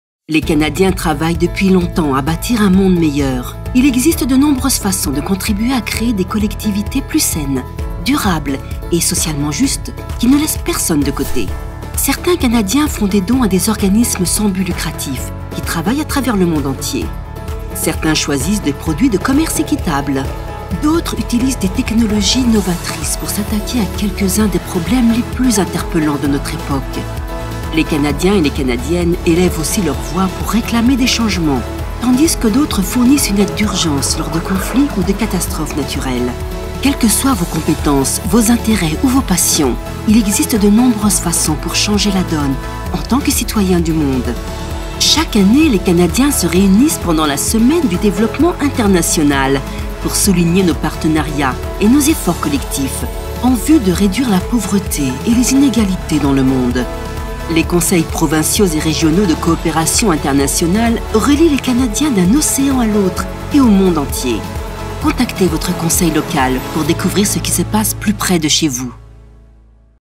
Qu'est-ce que la Semaine du développement international?
La Semaine du développement international aura lieu le 2e à 8e février 2020.

Comment les canadiens et canadiennes contribuent à créer des collectivités plus saines, durables et socialement justes, au niveau locale et globale?

La Semaine du développement international célebre et souligne nos partenariats et nos efforts collectifs en vue de réduire la pauvreté et les inégalités dans le monde.

Connectez avec les conseils provinciaux et régionaux de coopération internationale pour découvrir ce qui se passe plus près de vous.